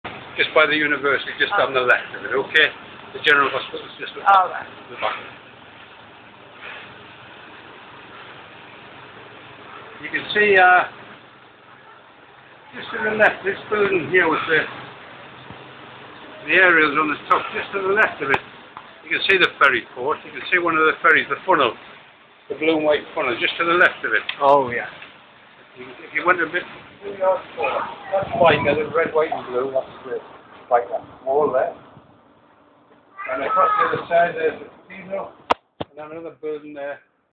Highest point of St James Park Newcastle
The view from the highest point of St James park Newcastle. A nice view of the city's skyline.